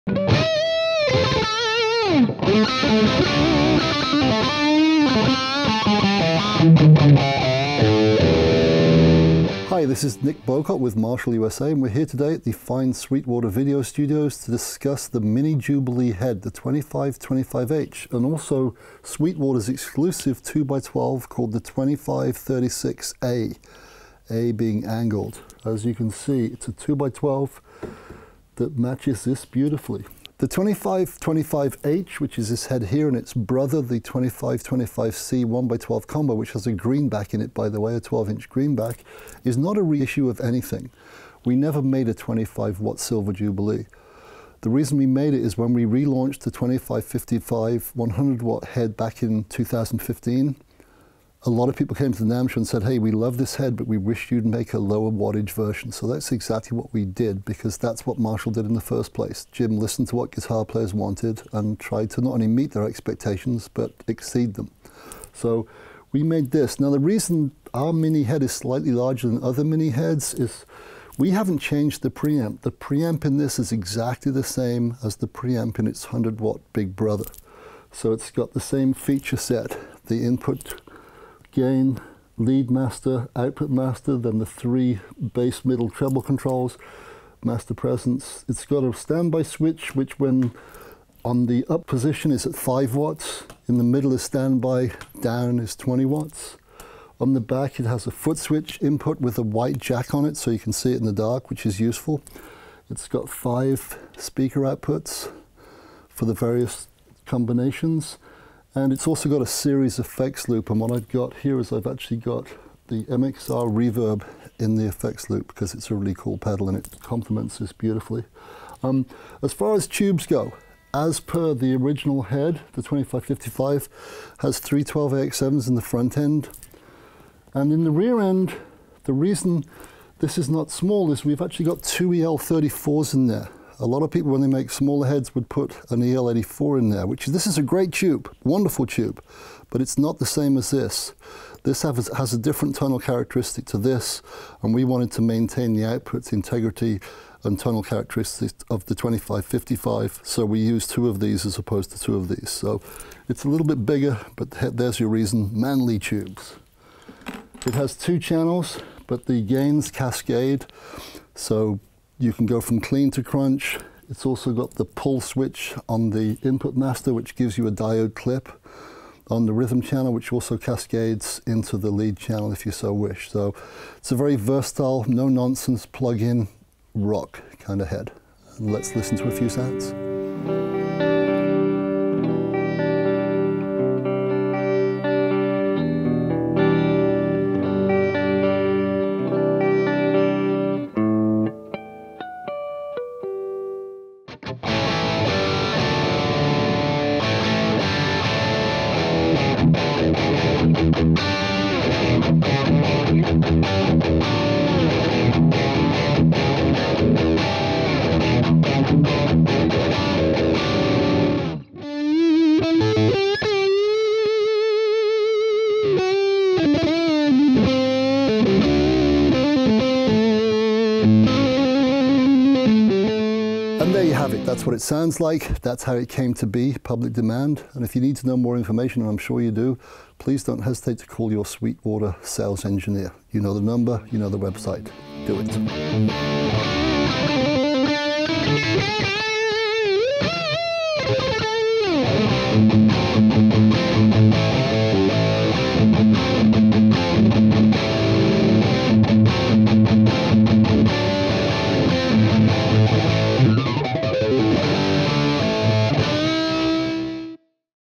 Marshall 2525H Mini Silver Jubilee Amp Head and 2536 Silver Jubilee Cab Demo
Nick Bowcott from Marshall presents the 2525H Mini Silver Jubilee amp head, along with its matching 2536A cabinet. This 20-watt 2525H Mini Silver Jubilee gives you everything you love about Marshall's highly coveted 2-channel, 100-watt head in a smaller and more volume-friendly package. The 2525H has all the tone and look of the original, like high- and low-output switching for 20- and 5-watt performance. This offers you the sound of a Marshall at full roar, but at studio and club levels. The Marshall 20-watt 2525H Silver Jubilee amplifier also gives you even lower noise performance and improved reliability over the 1980s originals.